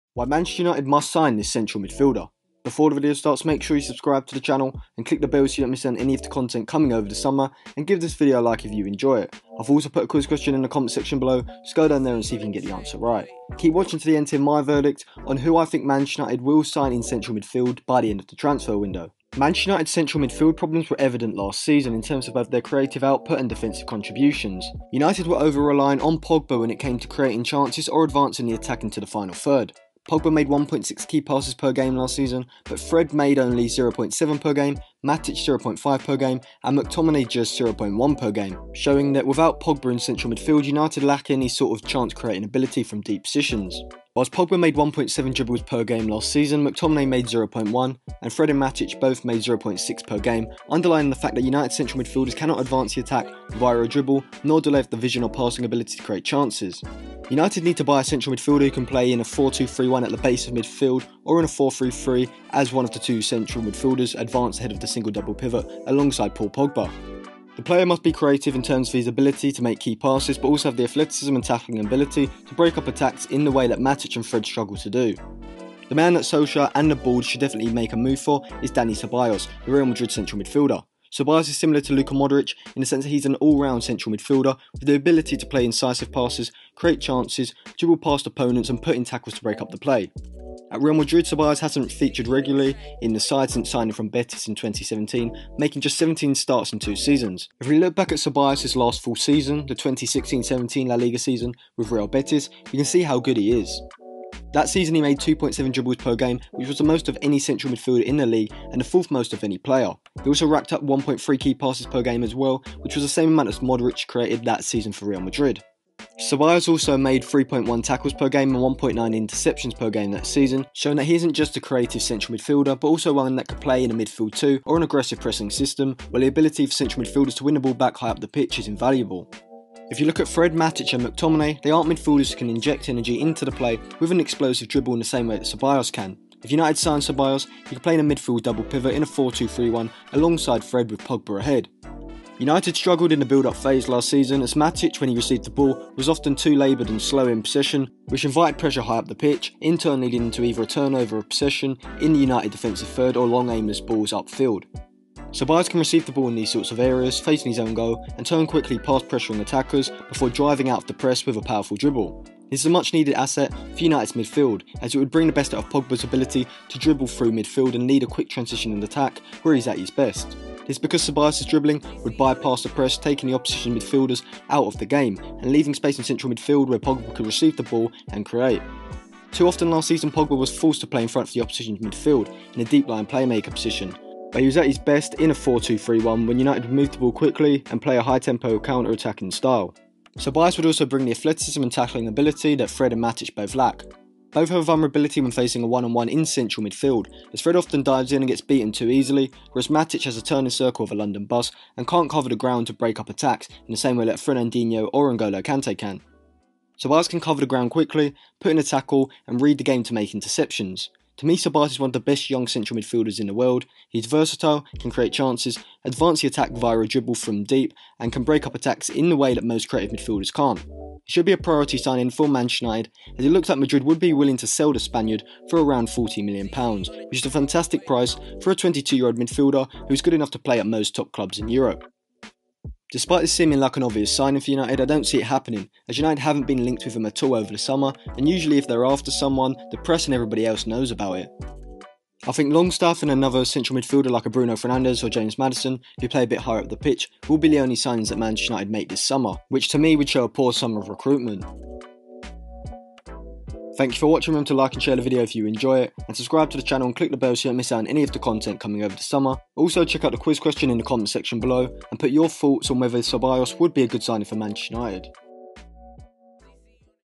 The Central Midfielder Man Utd MUST SIGN!! | Man Utd Transfer news 2019
An analysis of why Man Utd should sign this Real Madrid central midfielder to play alongside Paul Pogba. This player is Dani Ceballos who will cost around £40m. Man Utd have been linked with many midfielders over the transfer window this summer 2019, but this video details why Ceballos would be the perfect midfield option for Man Utd to sign.